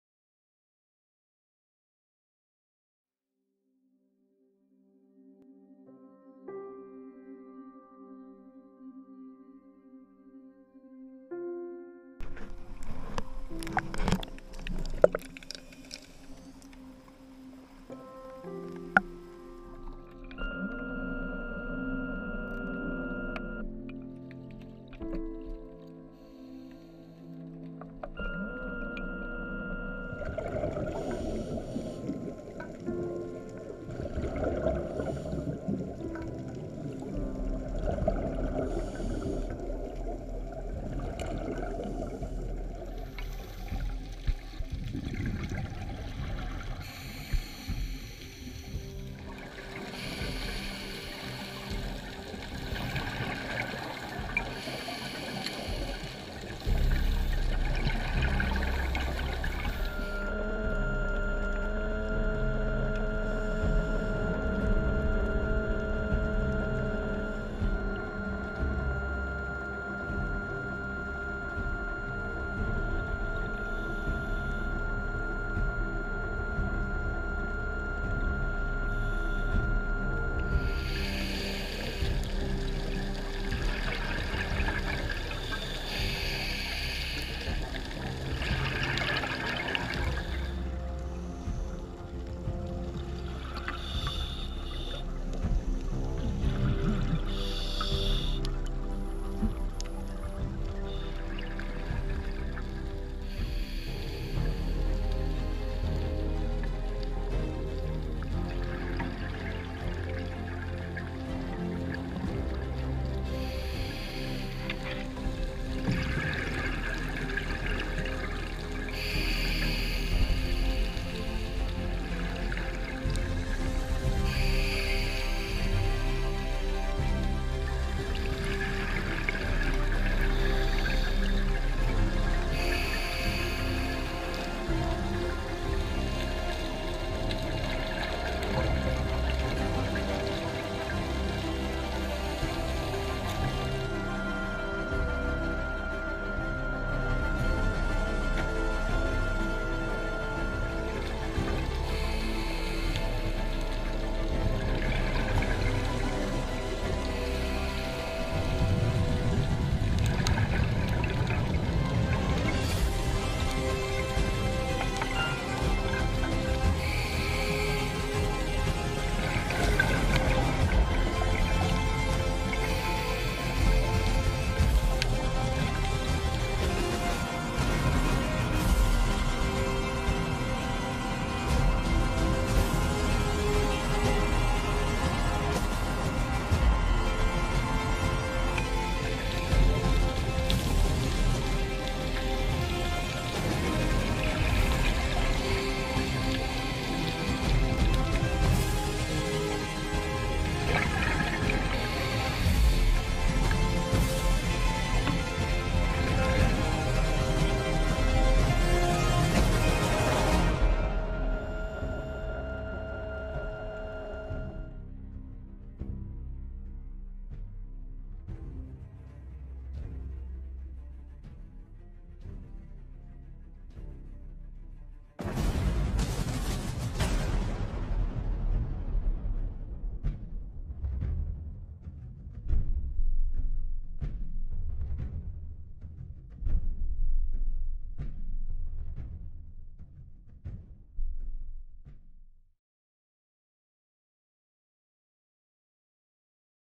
Long Scooter Dive to Beto's and GPW
A great summer dive with Ben and Ted. Doubles, stages, scooters, and a hijacked float (thanks random Lobos divers :)